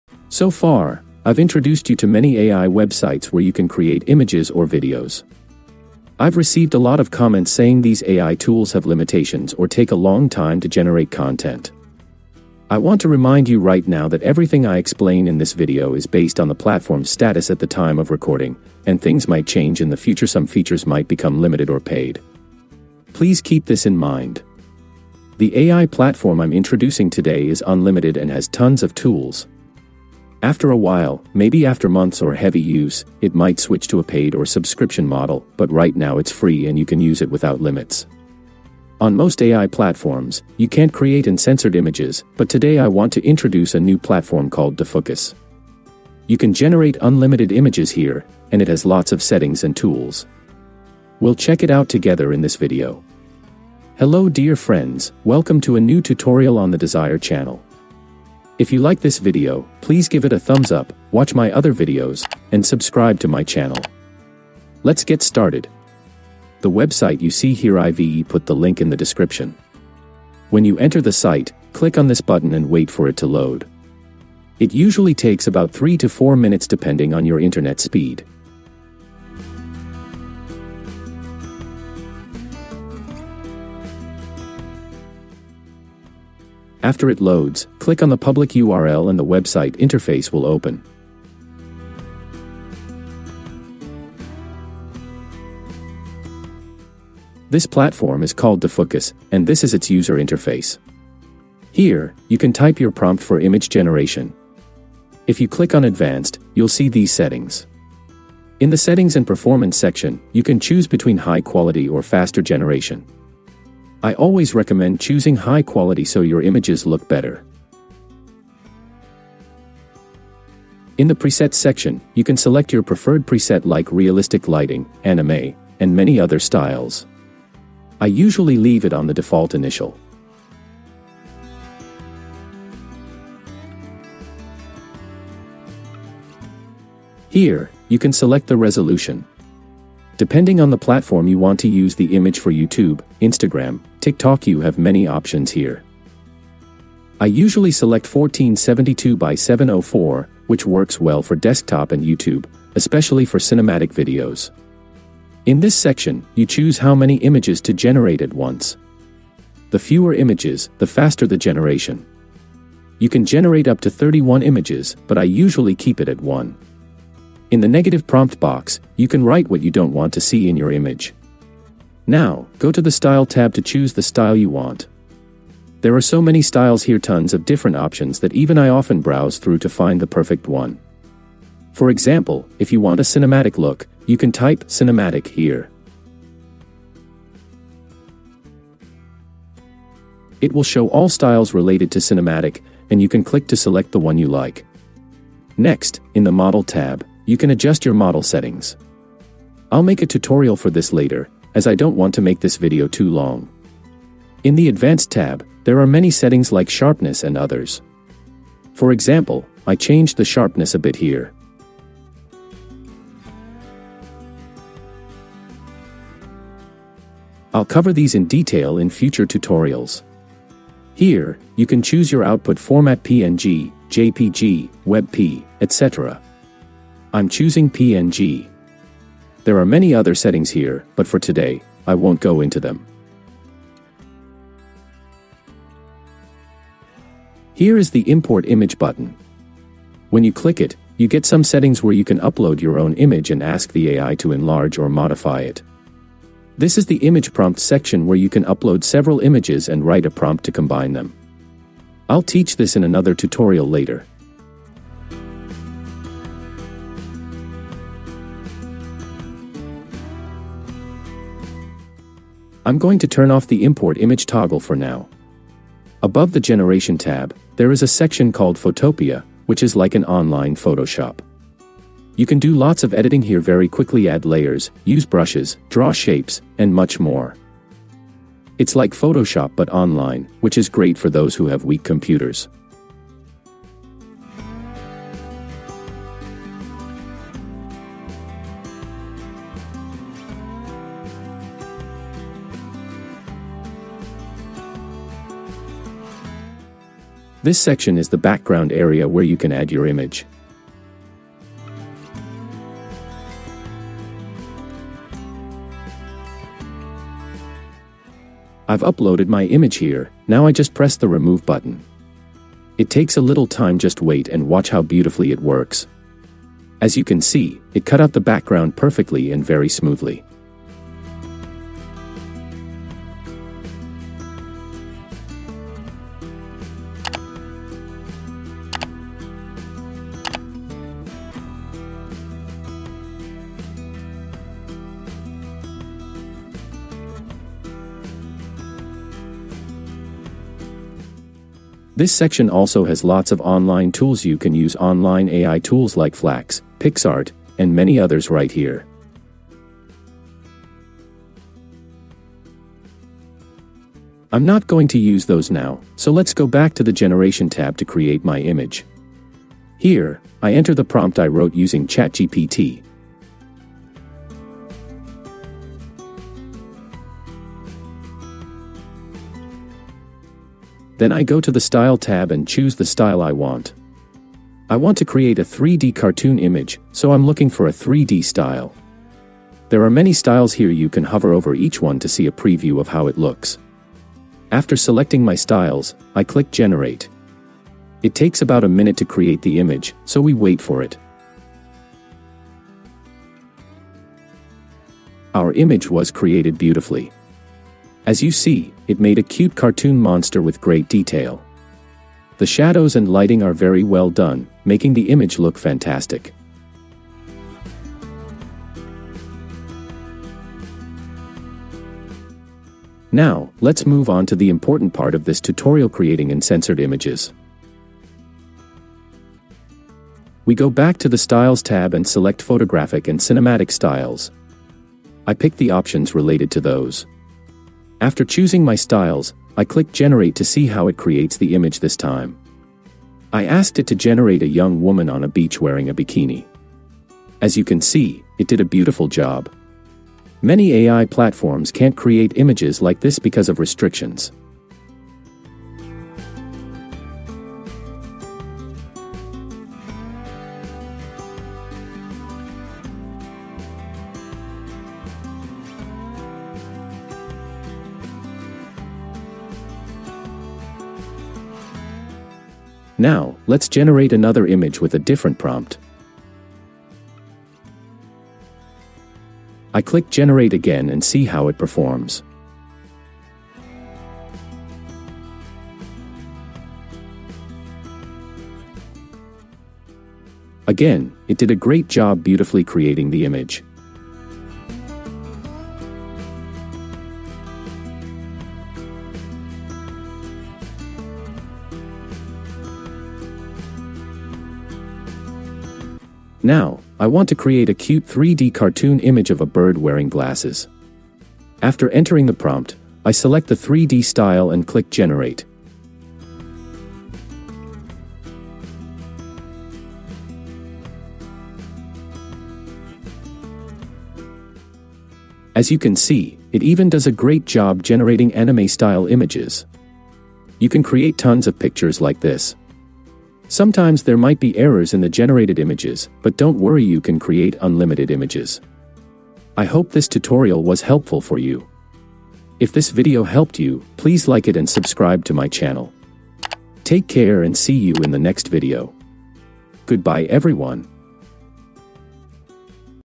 Create Stunning AI Images with DeFooocus | Unlimited, Free & Uncensored Tool
Create Unlimited, Free & Uncensored AI Images with DeFooocus | Easy Text-to-Image Tutorial
Free & Unlimited AI Art: Get unlimited free images
Multiple AI Models: Supports multiple Stable Diffusion models (SDXL, SD1.5, etc.) for varied styles and high-res output
Open-Source & Easy: Built on open-source Fooocus
 with a simple browser interface – no installation required
No Censorship: Create any image you want – DeFooocus imposes no strict content filters, so you have total creative freedom.
Extra AI Tools: Includes an Image Extender and Upscaler to enhance or enlarge your images with one click.
In this video, you will learn how to:
🚀 Access DeFooocus  and get   free image generations per day—plus how to unlock extra credits for unlimited use([Tom's Guide]
🎨 Craft diverse styles (Realistic, Anime, 3D Cartoon, Cinematic) using advanced presets and model settings
🔥 Generate uncensored content (e.g., beach scenes, mature themes) that most AI platforms block.
🛠️ Use bonus tools** like background removal, upscaler, and the built-in Photopea editor for final touches.

Why DeFooocus?
Free & Unlimited: Completely free right now, with no hidden paywalls—it may introduce subscriptions later, but you get unlimited today.
Online & Lightweight: Perfect for laptops or older PCs—no downloads needed.
Open-Source: Built on the stable diffusion codebase for transparency and community improvements.

Enjoying these AI tutorials?

Timestamps:
0:00 Introduction & Key Features
1:08 How to Start & Load DeFooocus
01:46 Advanced Settings & Presets
05:03 Background Removal & Photopea Editor
06:41 Generating 3D Cartoon & Cinematic Images
06:53 Creating uncensored images with AI